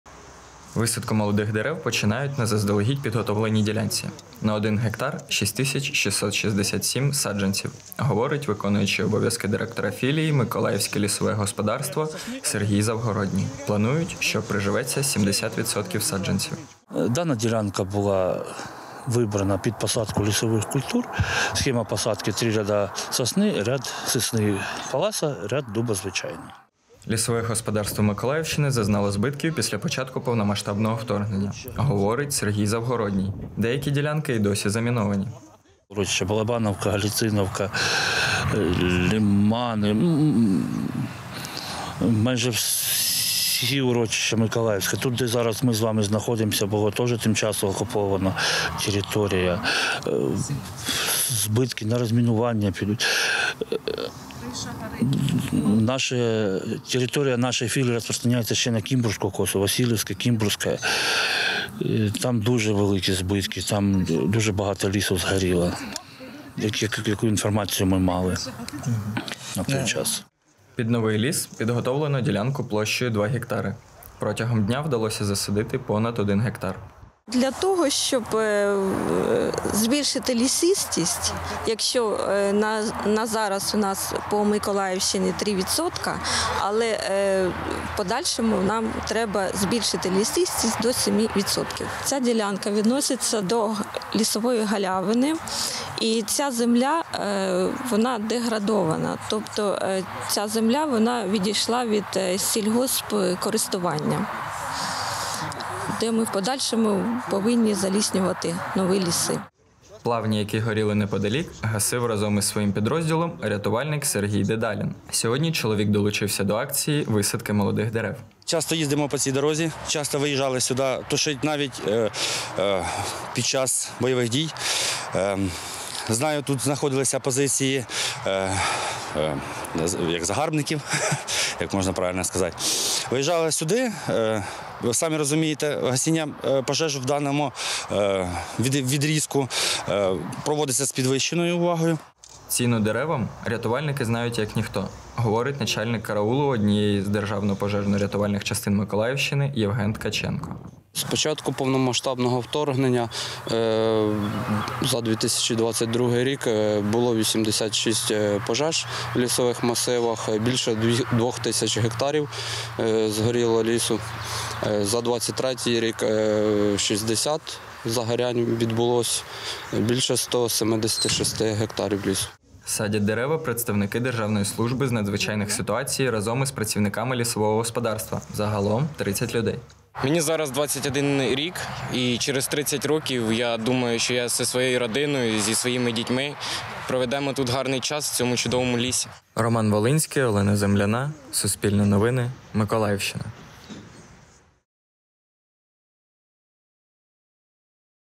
На Миколаївщині висадили сосни та дуби
Майже 7 тисяч садженців сосен та дубів висадили 21 березня у Михайло-Ларинській рощі представники лісового господарства разом із рятувальниками Миколаївщини. Акція відбулась в рамках програми президента України "Зелена країна".

Автор: Роман Волинський
Оператор: Олена Земляна